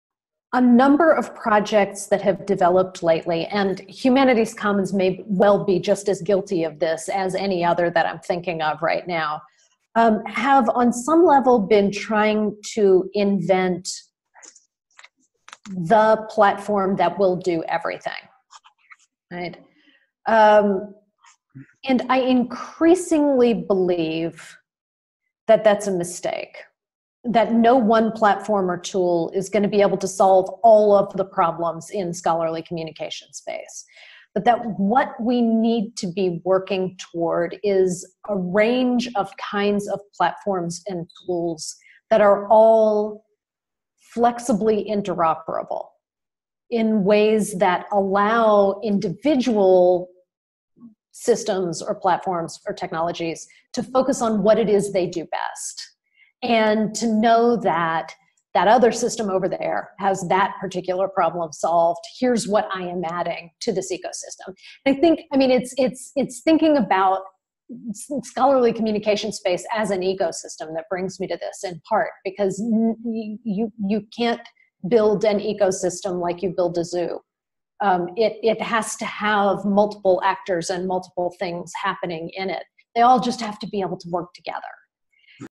Kathleen Fitzpatrick interview: Systems out of step
If we now see that too much is invested in the process and product of journals, one caution in response, from Kathleen Fitzgerald, is to avoid trying to solve all problems with one new platform.